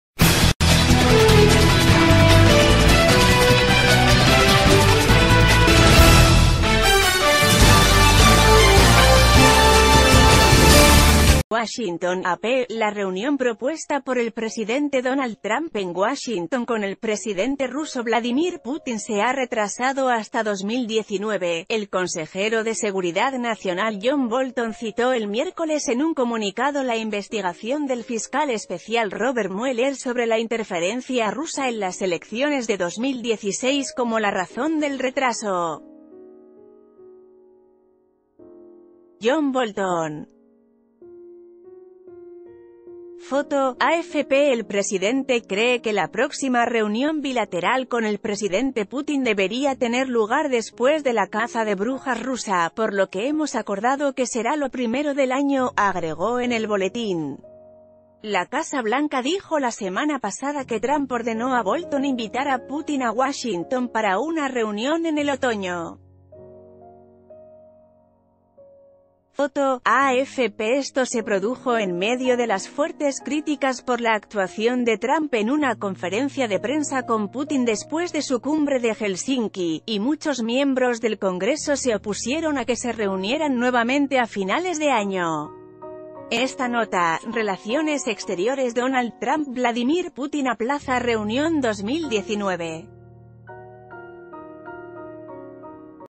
Próxima reunión entre Putin y Trump se aplaza hasta 2019
WASHINGTON (AP) — La reunión propuesta por el presidente Donald Trump en Washington con el presidente ruso Vladimir Putin se ha retrasado hasta 2019.El consejero de seguridad nacional John Bolton citó el miércoles en un comunicado la investigación del fiscal especial Robert Mueller sobre la interfer...